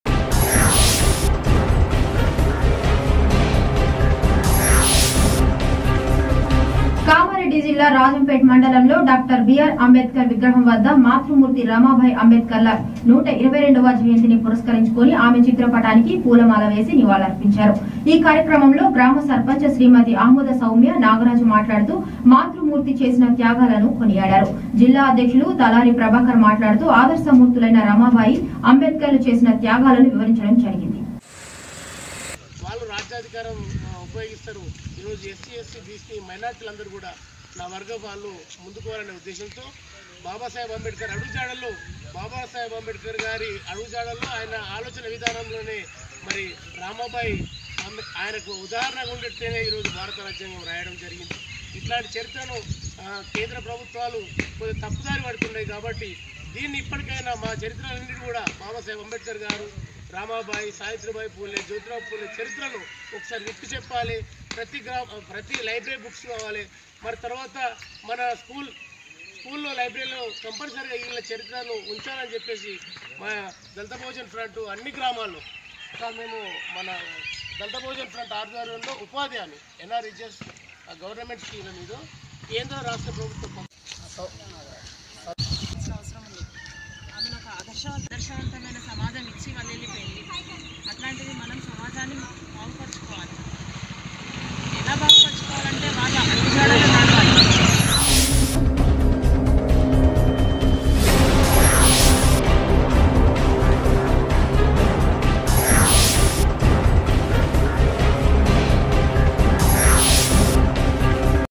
రాజంపేట్ మండలంలో డా. బి. ఆర్.అంబేద్కర్ విగ్రహం వద్ద మాతృమూర్తి రమభాయి అంబేద్కర్ ల 122 వ జయంతి //
కామారెడ్డి జిల్లా : రాజంపేట్ మండలంలో డా. బి. ఆర్.అంబేద్కర్ విగ్రహం వద్ద మాతృమూర్తి రమభాయి అంబేద్కర్ ల 122 వ జయంతిని పురస్కరించుకుని ఆమె చిత్ర పటానికి పూలమాల వేసి నివాళులు అర్పించారు. ఈ కార్యక్రమంలో గ్రామ సర్పంచ్ శ్రీమతి ఆముద సౌమ్య నాగరాజు మాట్లాడుతూ  మాతృ మూర్తి చేసిన త్యాగాలను కొనియాడారు. జిల్లా అధ్యక్షులు తలారి ప్రభాకర్ మాట్లాడుతూ ఆదర్శ మూర్తులైన రామభాయి  అంబేద్కర్ లు  చేసిన త్యాగాలను వివరించడం జరిగింది . . ఈ కార్యక్రమంలో ఉపసర్పంచ్ శివకుమార్,జిల్లా ప్రధాన కార్యదర్శి బత్తుల రాజేందర్, కార్యదర్శి ఏసుదాస్, జిల్లా నాయకులు గంగారాం , వార్డు సభ్యులు  లక్ష్మీ సత్యనారాయణ, అంబేద్కర్ సంఘము అధ్యక్షుడు కర్రోళ్ల రవి,  ప్రసాద్, కుమార్, , రంజిత్, నవీన్ తదితర దళిత బహుజన సంఘాల నాయకులు పాల్గొన్నారు.